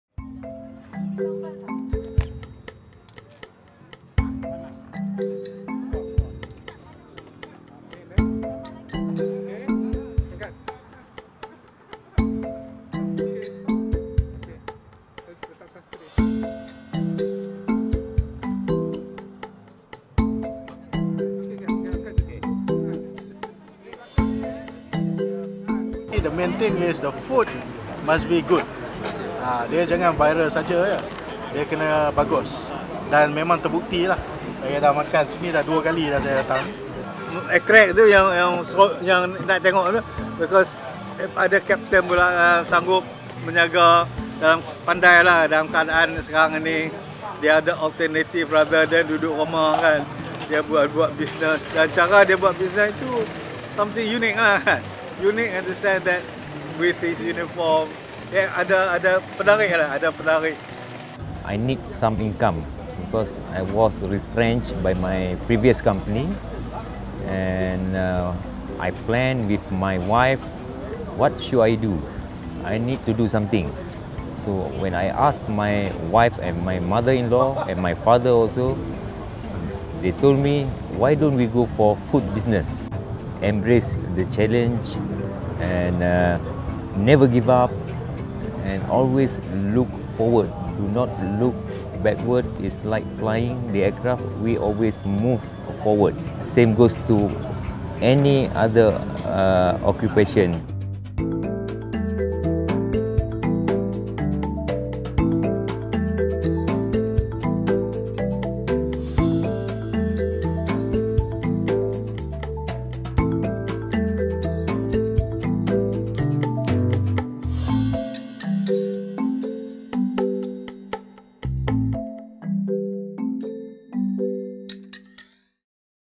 Former Malindo Air pilot turns cook
Every morning Malaysian aircraft pilot Captain Azrin Mohamad Zawawi dons his uniform and hat before heading to work - not at the airport, but to a small noodle stall that he now runs in Subang Jaya, Selangor.

Azrin is among the many Malaysians who lost their jobs during the Covid-19 pandemic.